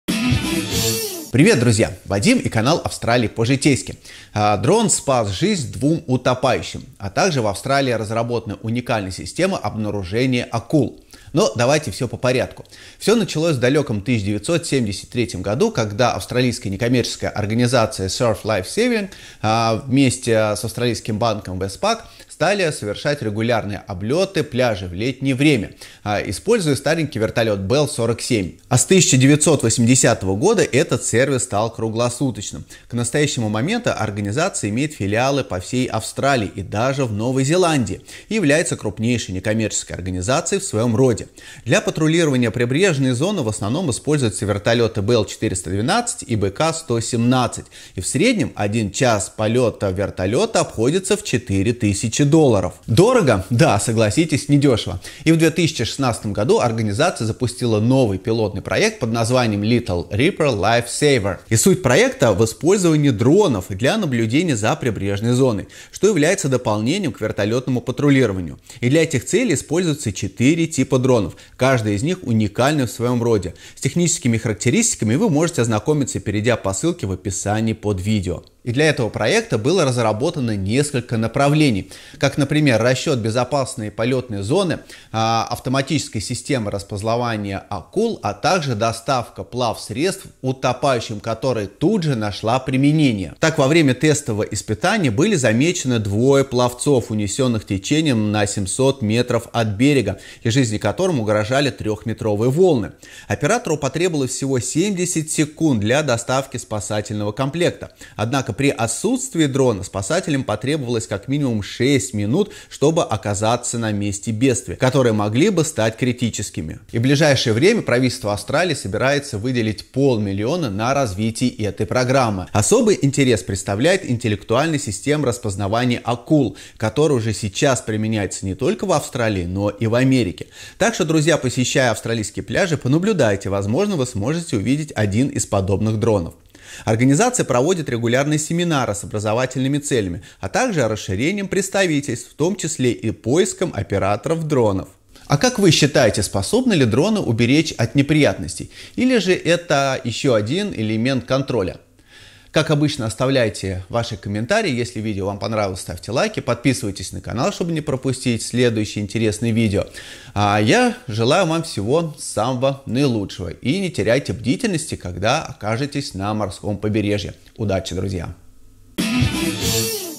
Акулы и дроны спасатели в Австралии
Использование дронов для спасения утопающих, обнаружение акул и многое другое
Westpac helicopter rescue  патрулирование пляжей и Little ripper life saver
Сделай сам свой переезд!!!